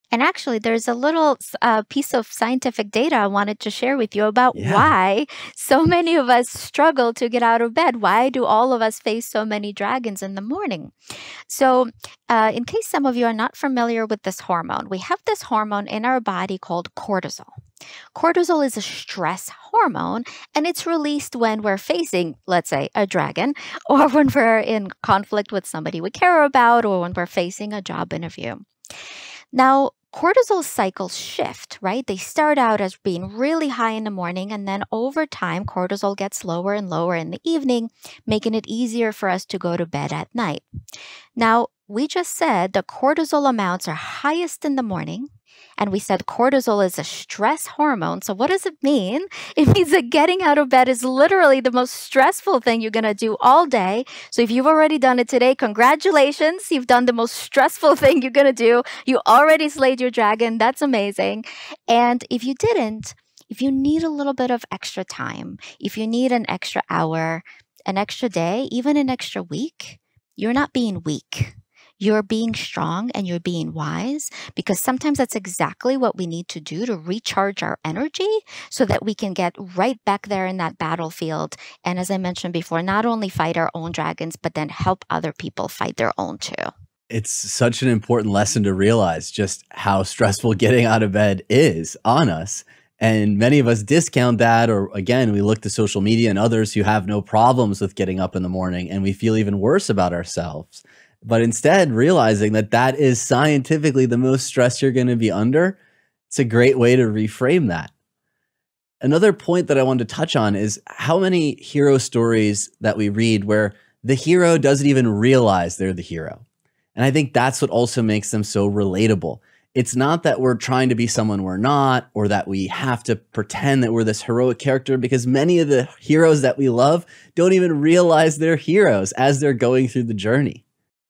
Too DEPRESSED To Get Out of Bed? - WATCH THIS | Dr. Janina Scarlet
The idea of finding a therapist and going to therapy can feel intimidating even though mental health is becoming less of a taboo subject. Some people get too depressed over suppressed emotions. Dr. Janina Scarlet, host of Superhero Therapy Podcast, talks about how getting too depressed is sometimes caused by the lack of self-awareness.